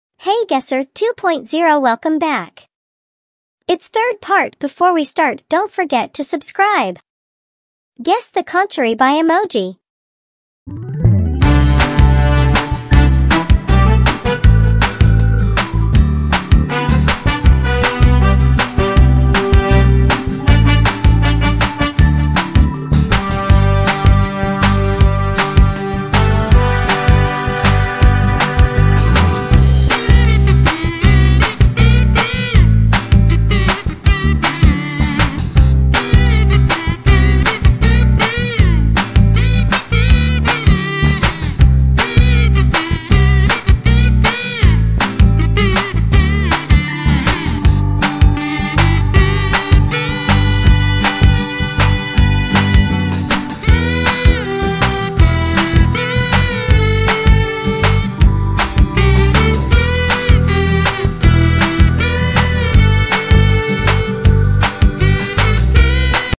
"Ultimate Guess the Country Quiz - Part 3 | Test Your Global Knowledge!"
hey guesser 2.0!
"Get ready for the most challenging installment of our 'Guess the Country Quiz' series with Part 3! Put your geography skills to the test and see how well you know the world's nations. Can you identify countries by their landmarks, flags, and more?